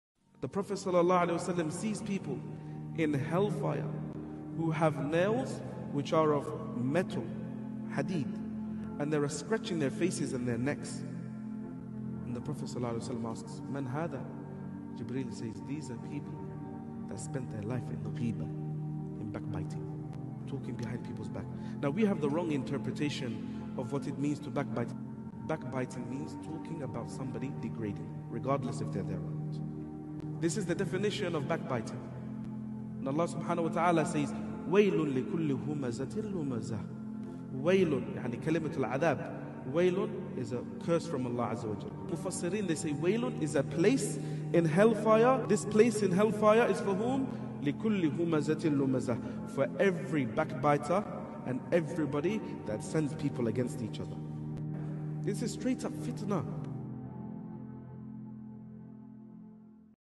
Punishment of Backbiting
This clip is an extract from the lecture below:
Al Isra Wal Miraj Un Nabi | WFIA Masjid Ghousia

Background Sound (Nuran Asani) @nuranasani1988